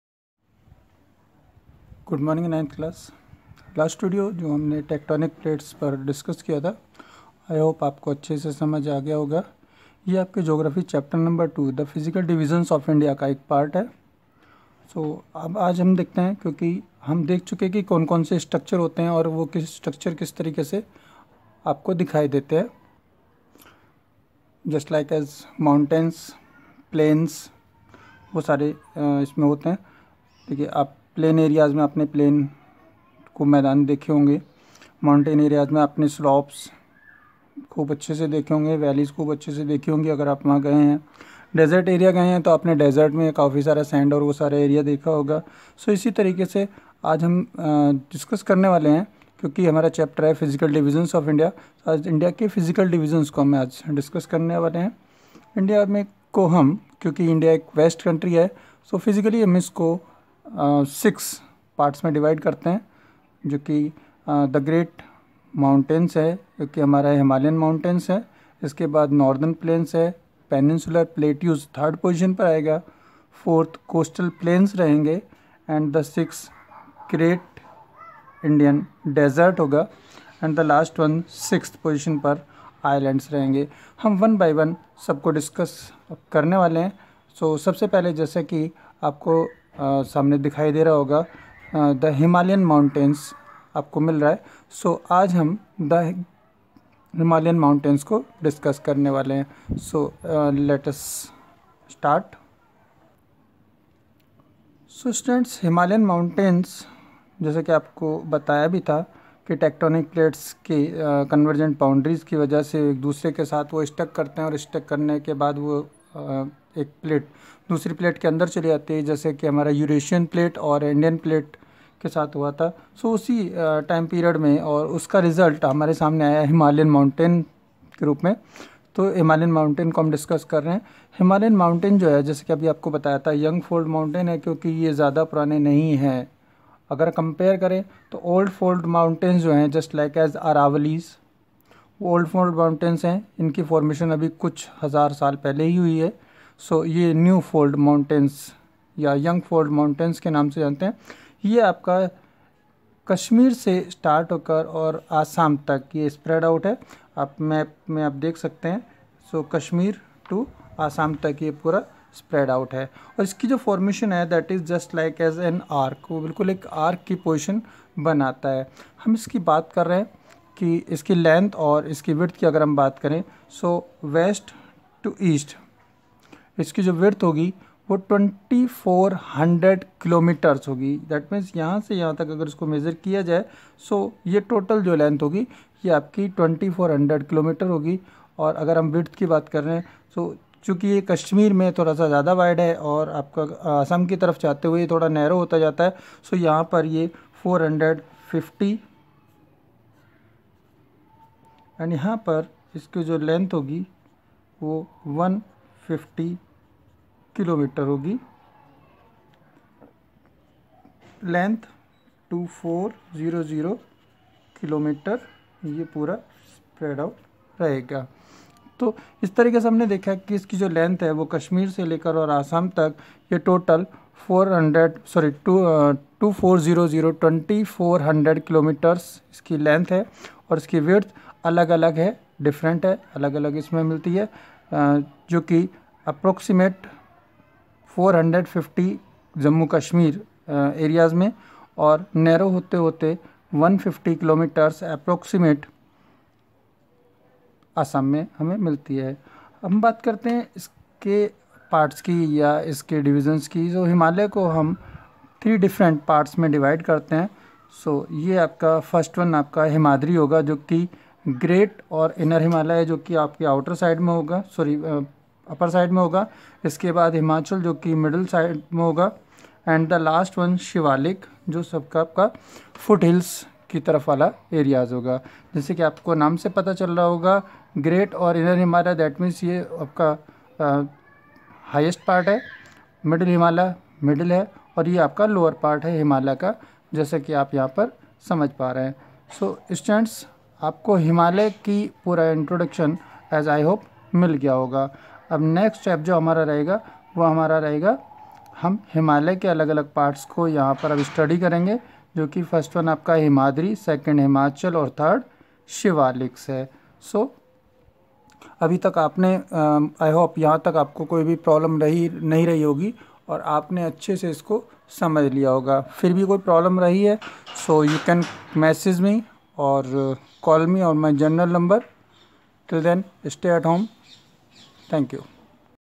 Intro -The Physical divisions of India &Himalayas. - 9th
In this video we will discuss about the physical division of India and the Himalayan mountains. - 9th